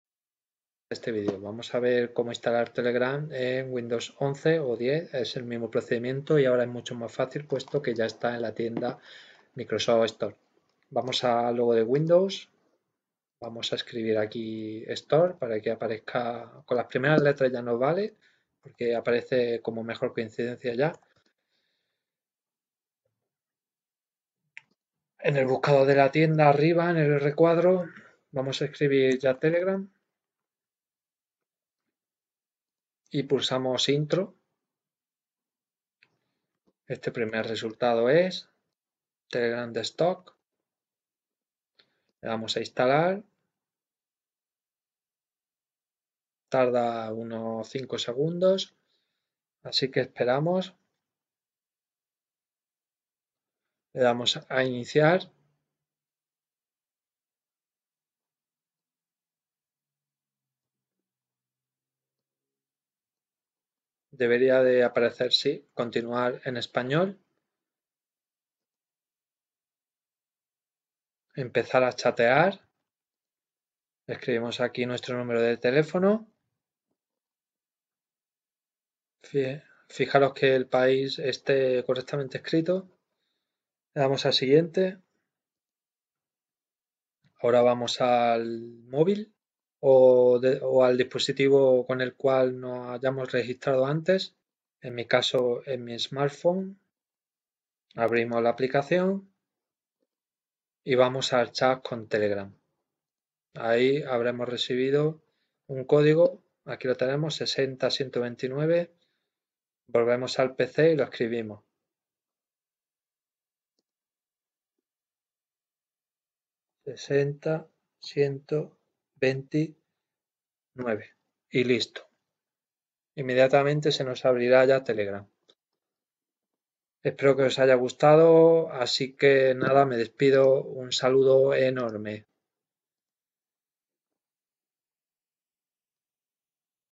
Descargar Telegram en pc Windows 11/10
¿Quieres saber cómo descargar Telegram en PC con Windows 11 o Windows 10?
Tutorial en español en que te enseño como descargar gratis y en español, la aplicación Telegram. Te explico cómo funciona el método de descarga desde la Microsoft Store, de la mejor alternativa posiblemente a Whatsapp, junto a Signal.